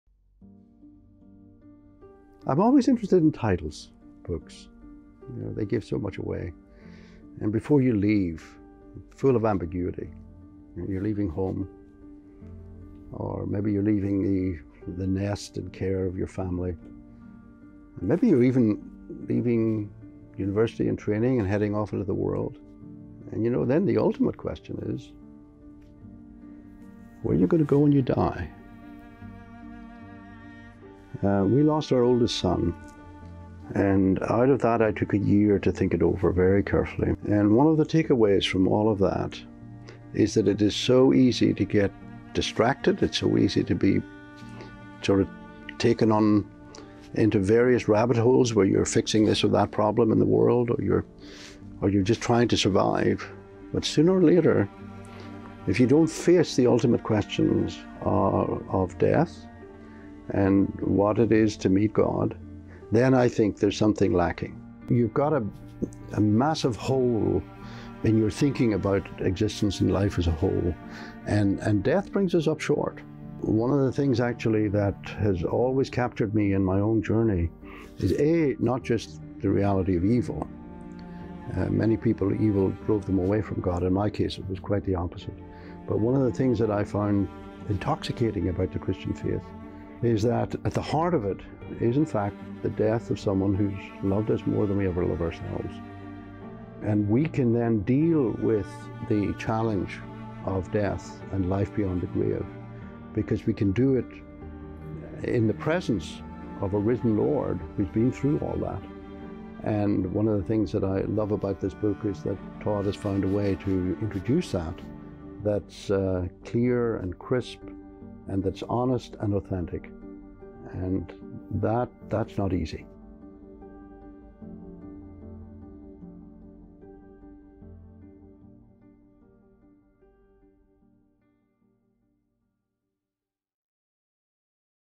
Before You Leave by Todd von Helms Trailer (with William J. Abraham)
Whether you are headed to college, starting your first job, or just reflecting on life, Before You Leave is written to help you understand and discuss challenging and essential questions of faith.

Dr. Todd von Helms has nearly two decades of insight gained through working with teenagers, college students, and clergy as an ordained minister, college professor, church consultant, research assistant, interviewer, chaplain and headmaster. He has degrees from the University of Texas, Dallas Baptist University, Southwestern Seminary, Southern Seminary, Duke University and Southern Methodist University. Today, Todd is a Senior Fellow at The King’s College in New York City, Presidential Scholar of Christianity and Culture at Dallas Baptist University, and a Senior Fellow for the Center for Faith and Culture at Southeastern Seminary in Wake Forest, NC.

This book will focus in on "Questions We Rarely Talk About—But All Ask"

The questions the book will be exploring:
CAN WE TRUST THE BIBLE?
HOW SHOULD WE THINK ABOUT ATHEISM?
IS SATAN REAL?
DOES GOD REALLY SEND PEOPLE TO HELL?
DOES PRAYER WORK?